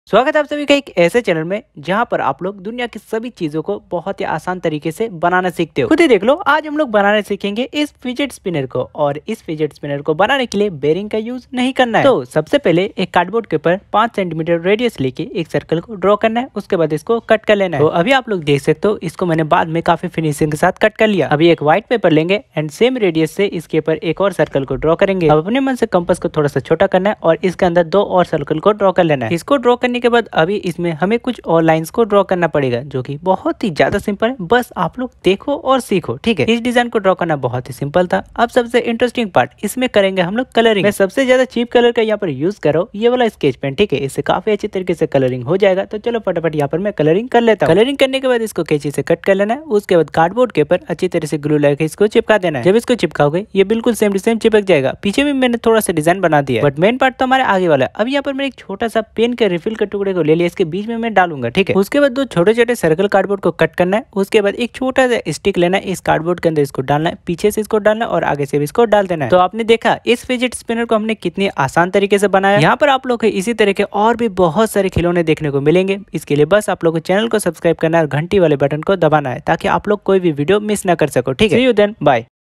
Cardboard Fidget Spinner ( Spin 60 Seconds )
In this video I'm going to make a fidget spinner out of cardboard and no use of bearing

Kuch baate is video ke baare me -
Aaj me aapko ek fidget spinner banake dikhaunga wo bhi cardboard se